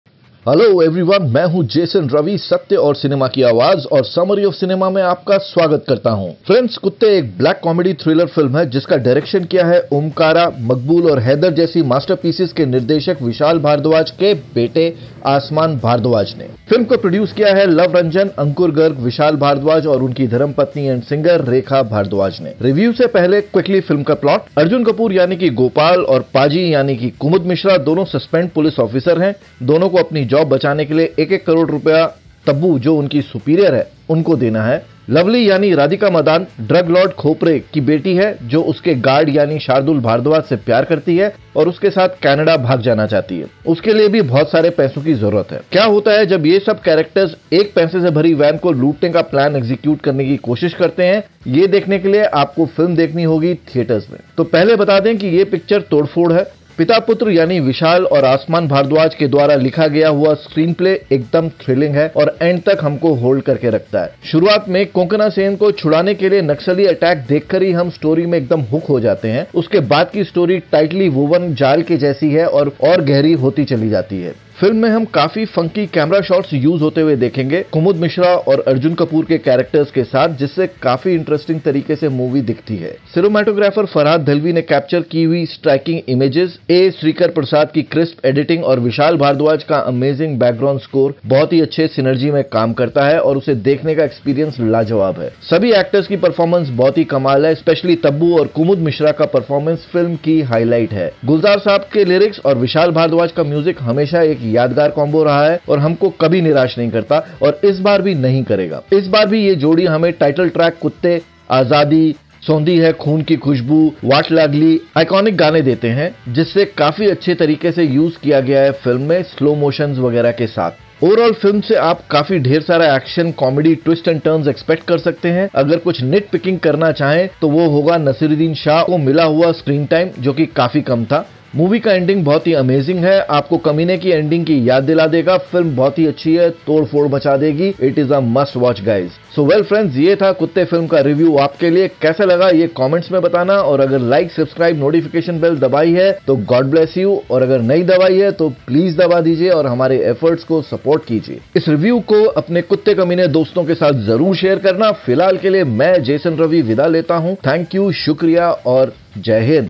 Kuttey Film Review in हिंदी I Summary of Cinema
A van carrying crores of cash. One rainy night in the outskirts of Mumbai. Unaware of each other, three stray gangs cross paths on the hunt. Unfortunately, all of them have the same plan. Bullets... Blood... Betrayal... It’s every man for himself... All the dogs after one bone. Will these dogs bite the bone, or will they lose to greed?

Kuttey Movie Review in Hindi By Summary of Cinema. Kuttey Movie Featuring Arjun Kapoor & Tabu is a Tribute to Shahid Kapoor Classic Kaminey, A Van is on the target in a Dark Scary Night with Many Dogs chasing it for many different reasons - A Villain vs Villain Suspense Thriller not to be Missed! Kuttey Full Movie Story Outline Explained & Positives Negatives would be shared in this Video. Strong Comeback of Bollywood in 2023 on the cards? Do leave your Public Review in the Comments Section.

Genre: Caper
Starring: Naseeruddin Shah, Tabu, Arjun Kapoor,  Konkona Sensharma, Kumud Mishra, Radhika Madan & Shardul Bhardwaj

Gulshan Kumar & Bhushan Kumar Present
A Luv Films & Vishal Bhardwaj Films Production

Kuttey
Director- Aasmaan Bhardwaj
Produced by- Luv Ranjan, Vishal Bhardwaj, Ankur Garg, Rekha Bhardwaj
Story & Screenplay - Aasmaan Bhardwaj
Dialogues & Additional Screenplay - Vishal Bhardwaj
Director of Photography - Farhad Ahmed Dehlvi
Editor - A. Sreekar Prasad
Lyrics - Gulzar
Music & Background Score - Vishal Bhardwaj
Production Designer - Subrata Chakraborty, Amit Ray
Costume Designer - Karishma Sharma
Action Director - Harpal Singh (Paali), Anton Moon
Sound Designer - Shajith Koyeri, Yash Darji
Casting Director - Mukesh Chabbra CSA